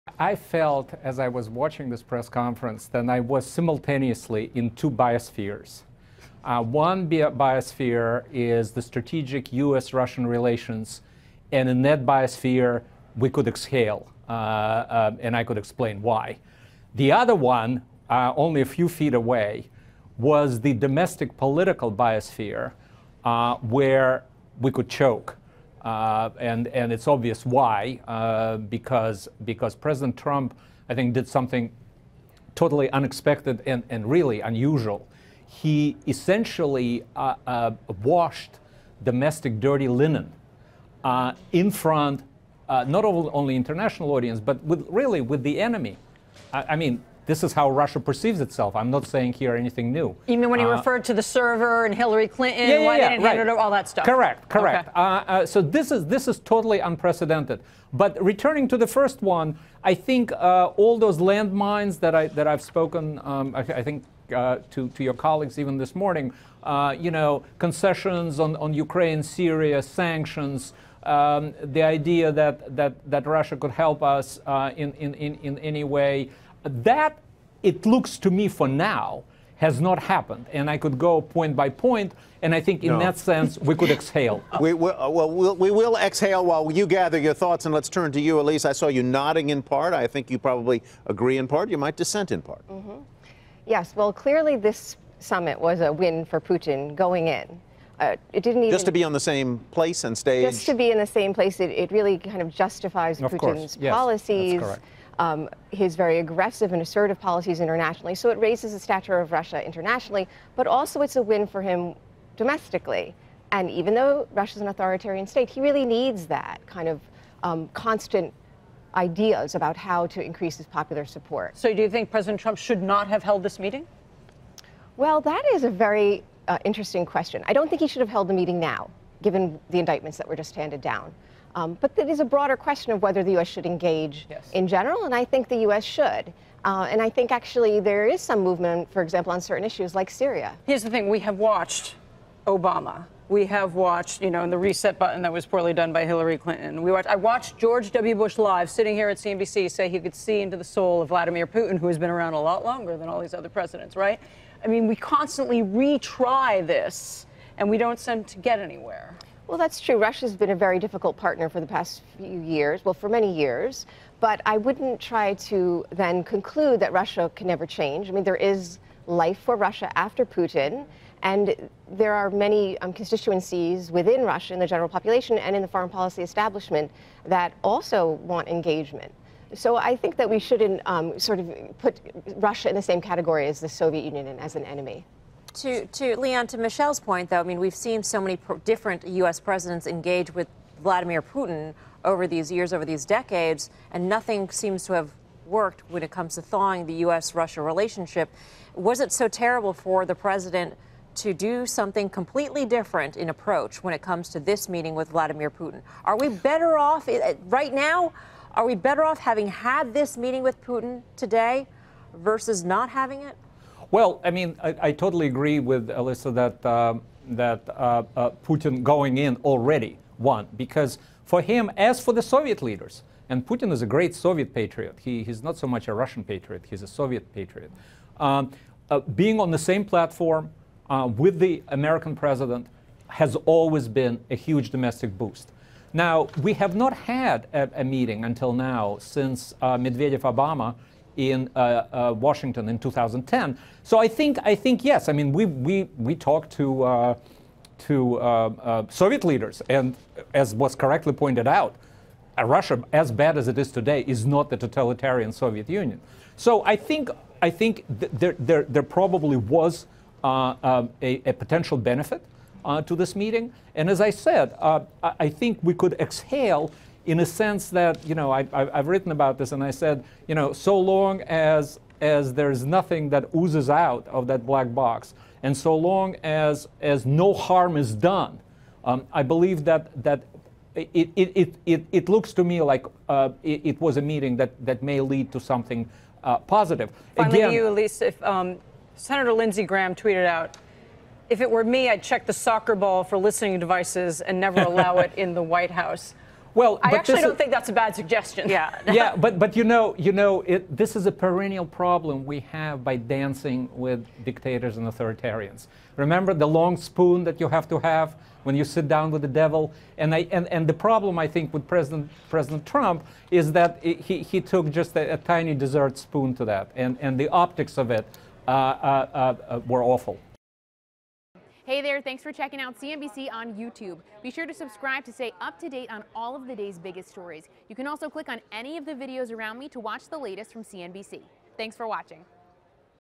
Trump washed domestic dirty linen in front of enemy, says expert
Elise Giuliano, Columbia University professor, and Leon Aron, American Enterprise Insitute director of Russian studies, discuss the press conference between Presidents Trump and Putin.